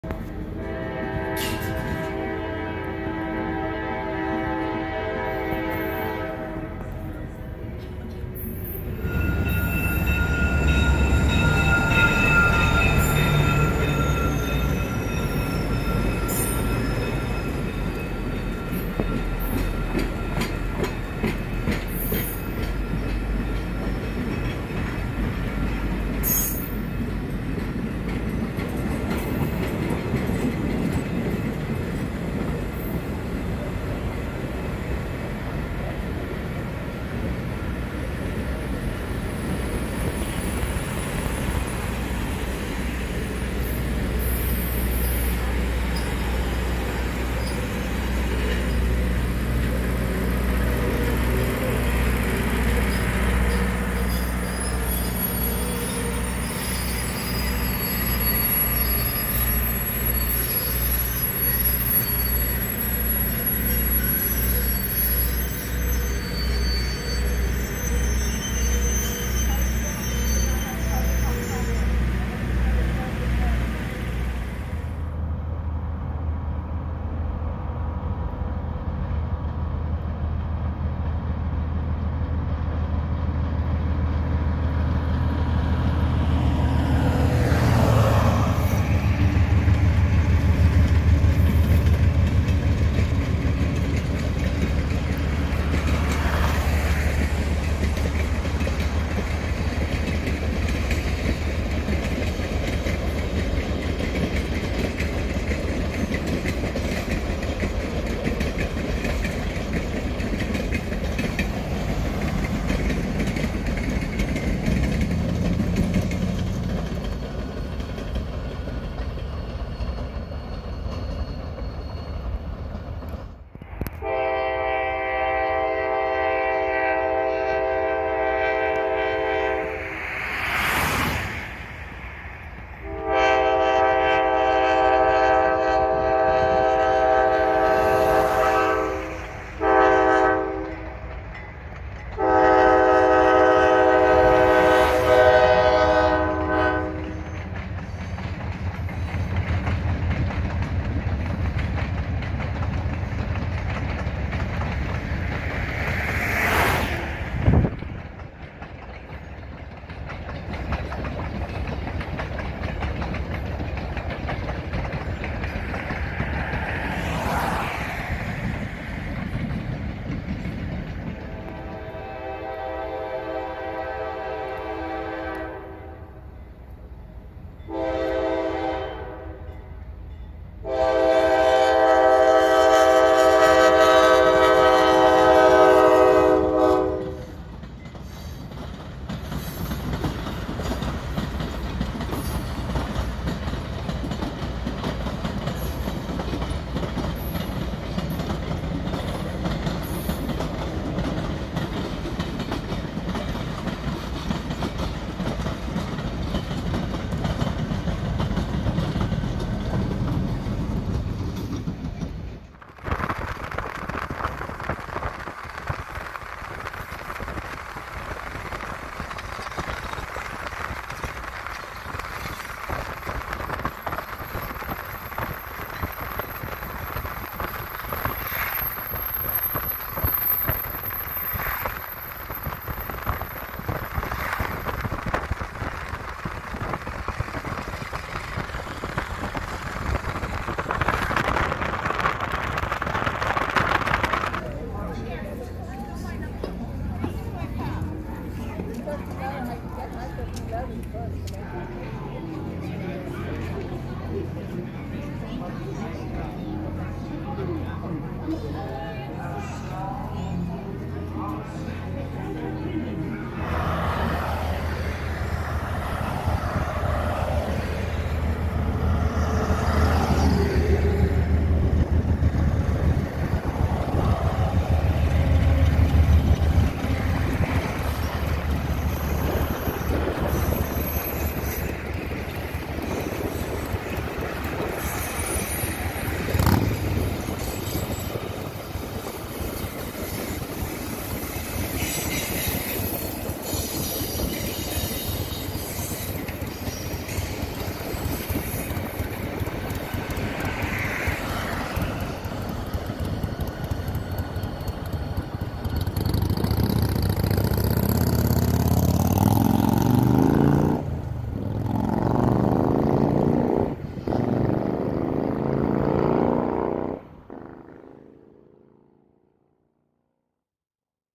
THE POLAR EXPRESS | THE DENNISON RAILROAD DEPOT MUSEUM
The Polar Express runs on the Ohio Central Railroad Mainline from Dennison Ohio, to the North Pole.